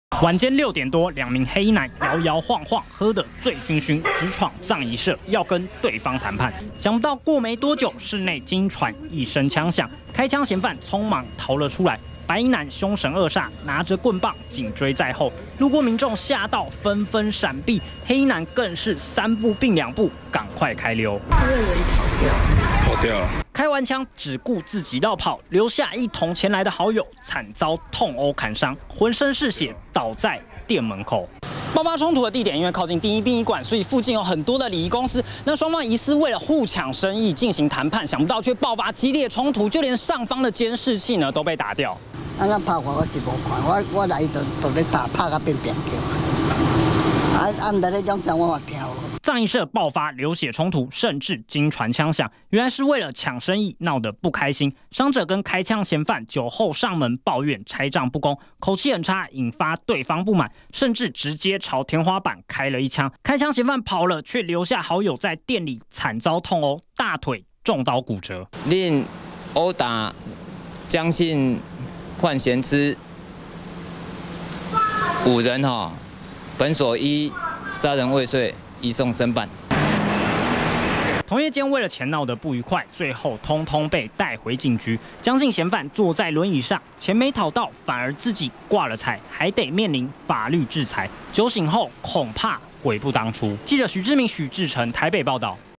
20180605中天新聞 拆帳談不攏 酒後揪友槍擊葬儀社反遭砍
北市民權東路上的一間葬儀社，突然傳出槍響，接著衝出一群黑衣人把開槍嫌犯打得落荒而逃，留下一名喝得醉醺醺江姓男子在葬儀社裡面，結果被打的頭破血流，大腿還被砍傷報復，幸好警方及時到場救人，才沒有爆發更嚴重的衝突。